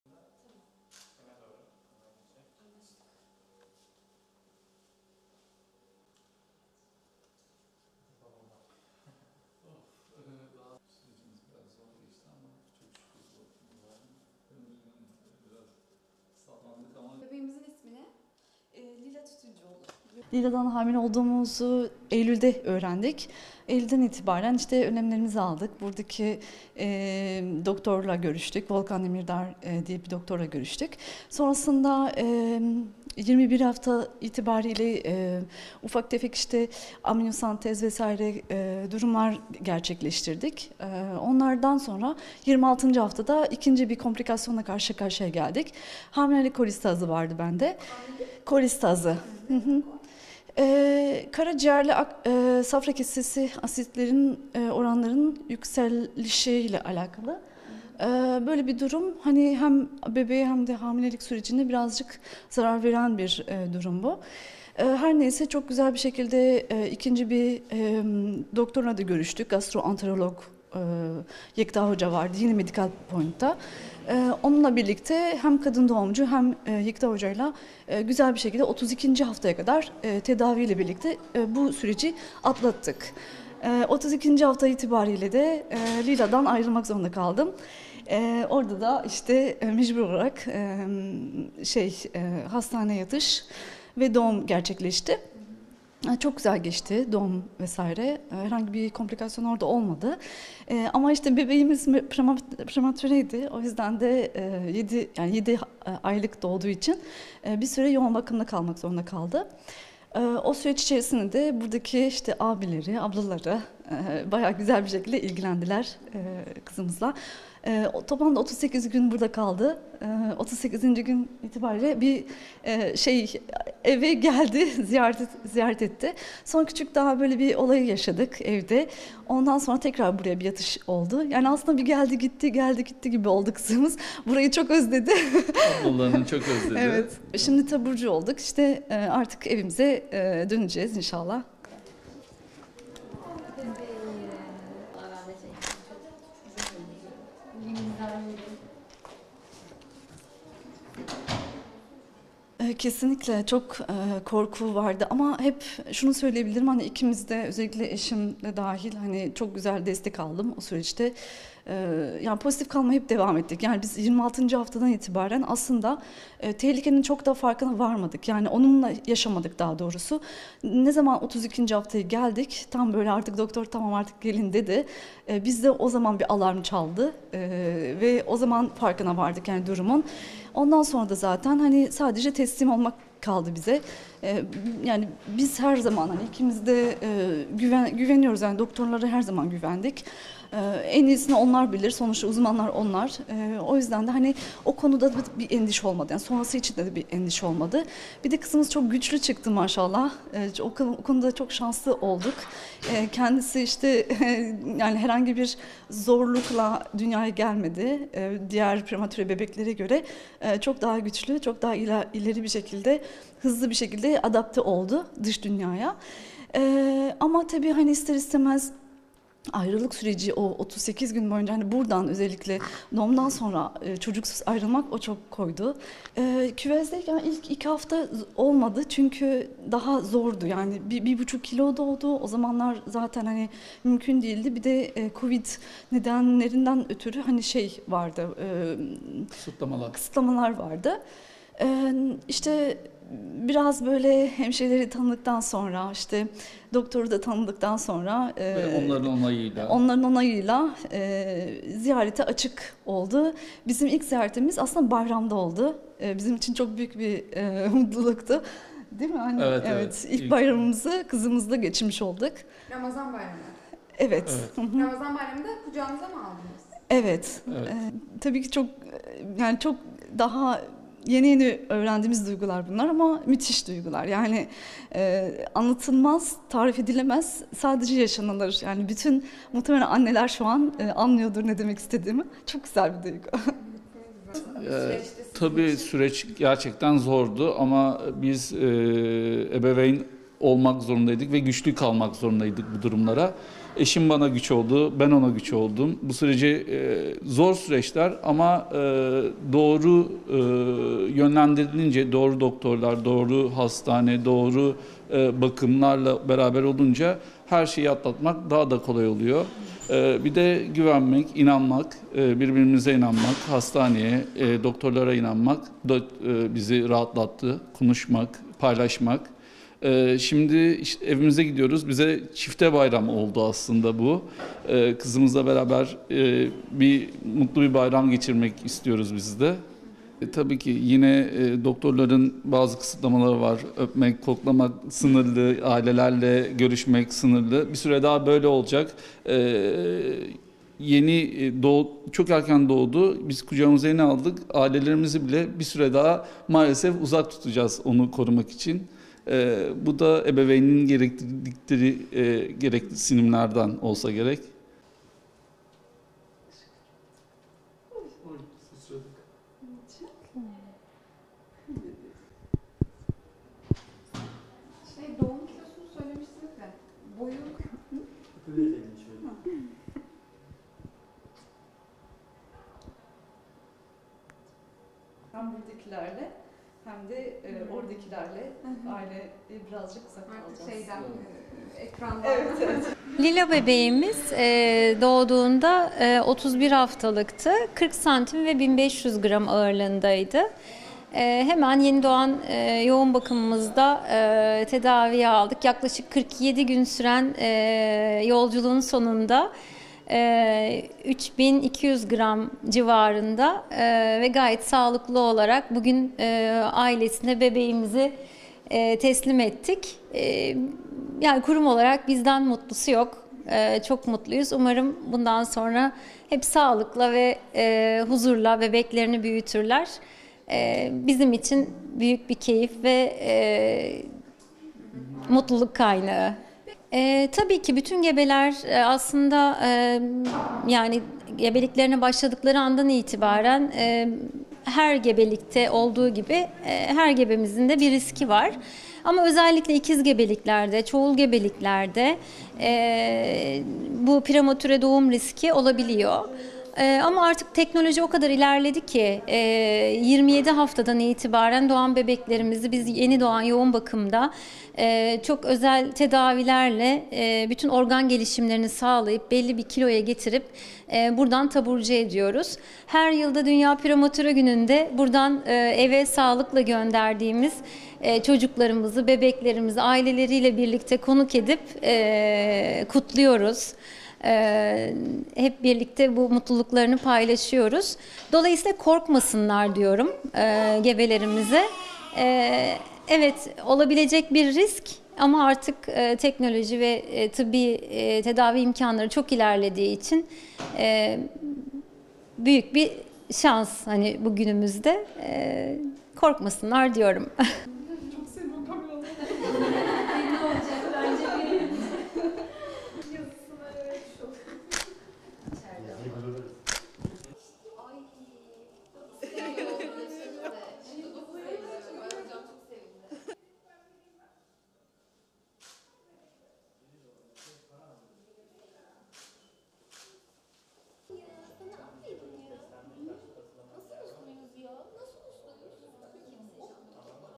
Lila bebek, bayramda kuvözden taburcu edildi
İzmir'in Aliağa ilçesinde yaşayan Zeliha Tütüncüoğlu (36) ve Melih Tütüncüoğlu (39) çiftinin 32'nci haftada dünyaya gelen ilk kızları Lila Tütüncüoğlu, toplam 47 gün kuvözde kaldıktan sonra ailesine kavuştu. Bebeklerini kucağına almanın mutluluğunu yaşayan genç çift, çifte bayram yaşadıklarını söyledi.

Aliağa'da özel bir firmada çalışan Melih Tütüncüoğlu ve Zeliha Tütüncüoğlu, geçen yıl eylül ayında bebeklerinin dünyaya geleceğini öğrendi. Çok istedikleri bebeklerine kavuşacağı için günleri saymaya başlayan Zeliha Tütüncüoğlu, 21 haftalıkken yapılan amniyosentezin ardından 26'ncı haftada hamileliği ile ilgili bir komplikasyon geliştiği bilgisini aldı. Karaciğer ve safra kesesi asit oranlarının yükselişi anlamına gelen hamilelik kolestazı yaşayan Tütüncüoğlu, 32'nci haftada Medical Point İzmir Hastanesi'nde bebeğini dünyaya getirdi. Doğduğunda 46 santimetre uzunluğunda, 1,5 kilogram ağırlığında olan Lila bebek, 47 gün süren tedavisinin ardından taburcu olup yeni bir hayata başladı.

'TESLİM OLDUK VE DOKTORLARIMIZA GÜVENDİK'

Bebeklerinin 2,5 aylık olduğunu ve 49 santimetre uzunluğa, 3 kilo 150 gram ağırlığa ulaştığını belirten Zeliha Tütüncüoğlu, yaşadıkları süreci şöyle anlattı:

"Hamileliğimde 32'nci haftaya kadar tedaviyle geldik. Ama 32'nci haftada Lila'dan ayrılmak zorunda kaldım. Doğum güzel geçti. 7 aylık doğduğu için bir süre yoğun bakımda kaldı. 38'inci gün çıktı eve geldi. Ama evde yeni bir rahatsızlık yaşayınca tekrar yatışı oldu. Toplam 47 gün kuvözde kaldı. Bu süreçte korkularım oldu. Ama eşimle birlikte çok iyi destek aldım. Hep pozitif kalmaya devam ettik. Teslim olduk ve doktorlarımıza güvendik. Kızımız çok güçlü çıktı maşallah bu konuda çok şanslıydık. Diğer prematüre bebeklere göre dış dünyaya hızlı adapte oldu. Bana en çok doğumdan sonra hastaneden çocuksuz ayrılmak koydu. Kuvözde kaldığı ilk 2 hafta hiç göremedim. Zamanla doktorlar ve hemşireler birbirimizi tanıdıktan sonra onu görmemize izin verdiler. Onu ilk ziyaretimiz Ramazan Bayramı'nda oldu. Kucağımıza aldık ama burada kalmaya devam etti. Önümüzde Kurban Bayramı var. Birlikte eve gideceğiz. Yeni yeni öğrendiğimiz duygular yaşıyoruz. Anlatılmaz, tarif edilemez, sadece yaşanır. Anneler beni anlıyordur. Çok güzel bir duygu."

'ÖPMEK, KOKLAMAK SINIRLI'

Ailece mutlu bir bayram geçirmek istediklerini ifade eden baba Melih Tütüncüoğlu da "Süreç zordu ama biz güçlü kalmak zorundaydık. Eşim bana güç oldu ben ona güç oldum. Doğru doktorlar ve hastaneye yönlendirilince her şeyi atlatmak daha kolay oluyor. Doktorlarımıza güvendik, evimize gidiyoruz. Bize çifte bayram oldu. Kızımızla beraber mutlu bir bayram geçirmek istiyoruz. Doktorların bazı kısıtlamaları var. Öpmek, koklamak sınırlı, ailelerle görüşmek sınırlı. Bir süre daha böyle olacak. Kucağımıza yeni aldık. Onu korumak için ailelerimizi bir süre daha uzak tutmak zorundayız" dedi.

'22 HAFTADAN İTİBAREN TÜM ORGAN GELİŞİMLERİ SAĞLANIYOR'

Medical Point İzmir Hastanesi Başhekim Yardımcısı Uzm. Dr. Özlem Günay ise son dönemde prematüre bebeklerde artış olsa da teknolojinin ilerlemesiyle onları sağlıklı bir şekilde ailelerine kavuşturduklarını ifade etti. Özellikle çoğul gebeliklerde riskin daha fazla olduğunu anlatan Günay, "Lila bebeğimiz doğduğunda hemen yenidoğan yoğun bakımda tedaviye aldık. 47 gün süren yolculuğun sonunda bebeği ailesine teslim ettik. Çok mutluyuz. Bundan sonra hep sağlıkla ve huzurla bebeklerini büyütmelerini diliyorum. Bütün gebelerde risk var. Özellikle ikiz ve çoğul gebeliklerde prematüre doğum riski olabiliyor. Teknoloji o kadar ilerledi ki 22 haftadan itibaren doğan bebeklerimizi yenidoğan yoğun bakımda çok özel tedavilerle tüm organ gelişimlerini sağlayıp belli bir kiloya getiriyoruz. Her yıl dünya prematüre gününde eve sağlıkla gönderdiğimiz bebekleri, çocukları aileleriyle birlikte konuk ediyoruz. Anne babalar korkmasınlar. Evet risk var. Ama teknoloji ve tıbbi tedavi imkanları çok ilerlediği için şansımız var" diye konuştu.

Nevra UÇKAÇ/İZMİR, (DHA)-